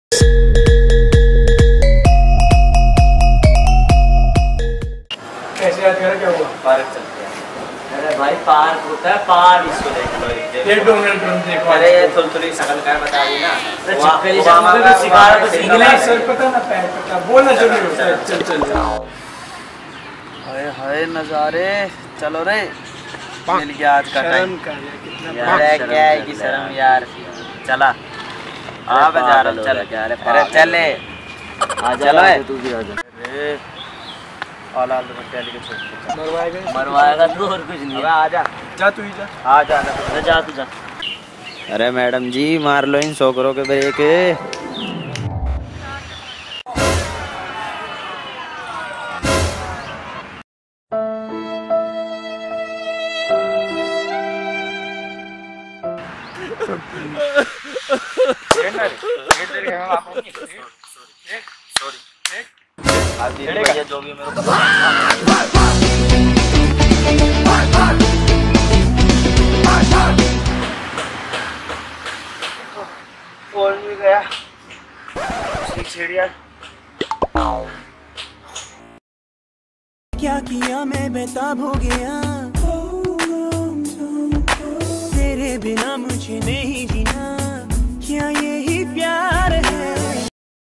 When Flirting Goes Wrong | Tokas Productions
"When Flirting Goes Wrong"

Casts - Ankit Tokas (Flirty Boy)
           - Prince Tiwari (Dont Know)
           - Sagar Tokas (Friend)
           - Jay Tokas (Friend "So Called Donald Trump)

Location - Munirka Village & R.K. Puram Sec. - 5 (Park)

Hope You Guys Like Our Team Work !!!